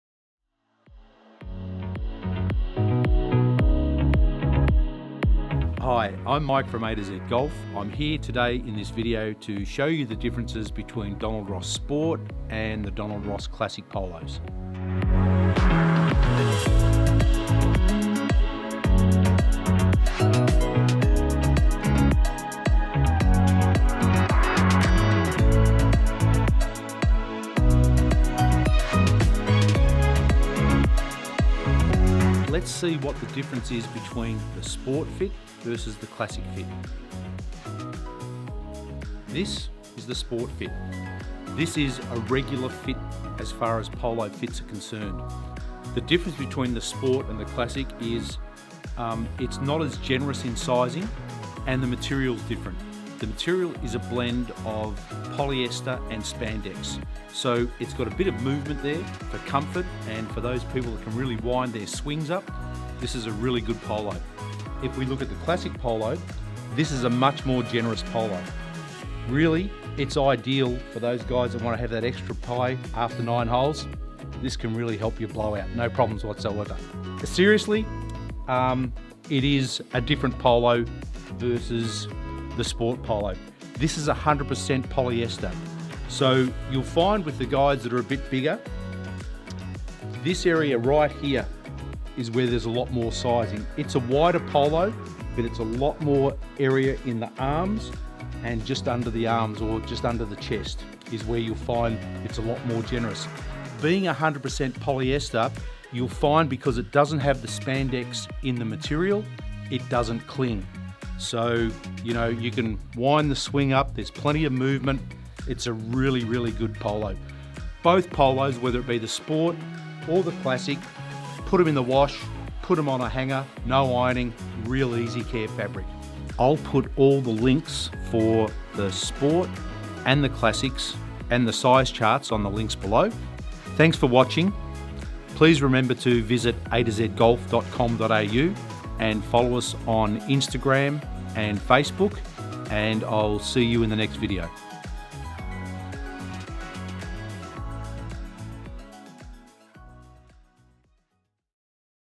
The Difference between DONALD ROSS Sport and Classic Polo Shirts | A2Z Golf
In this video Mike from A2Z Golf shows you the differences between Donald Ross Sport and Classic Polo Shirts.

DONALD ROSS SIZE CHART
Donald Ross Size Chart - COMING SOON